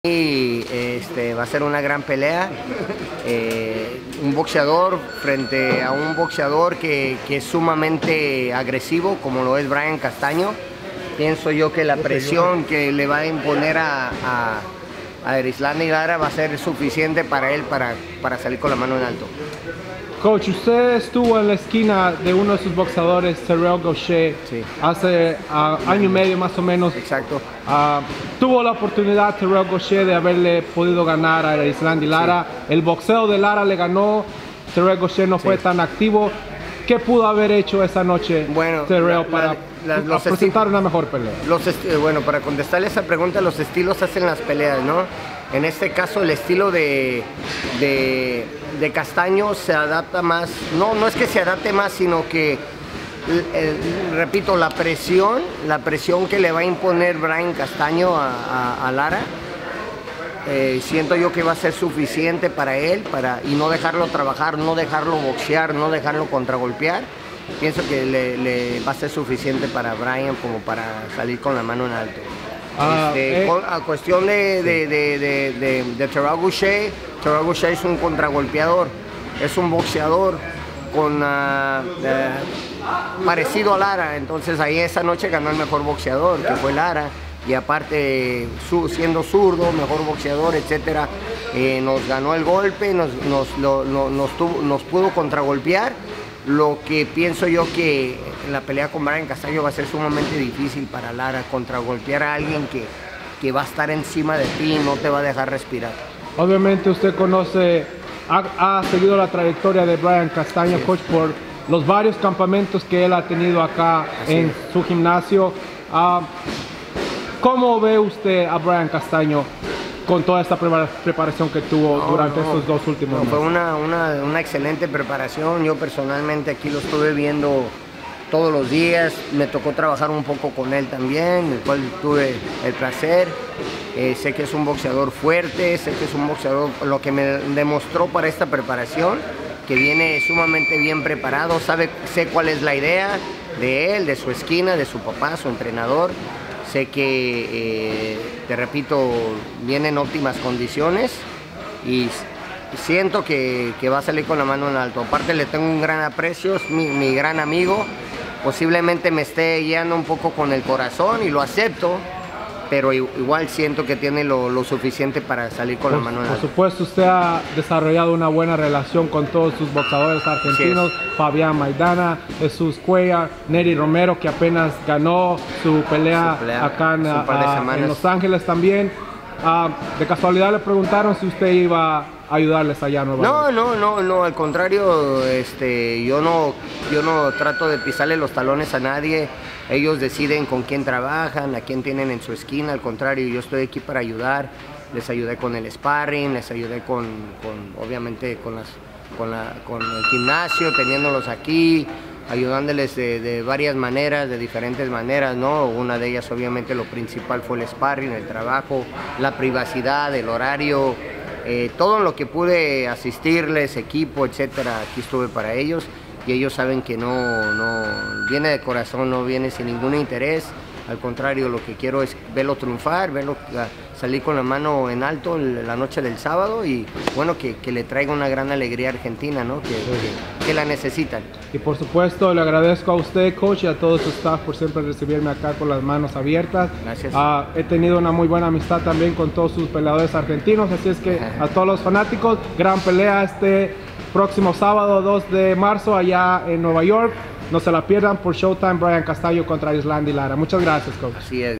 Coach Manny Robles analiza la pelea de Brian Castaño vs Erislandy Lara.
Legendz Boxing Gym.  Norwalk, Ca.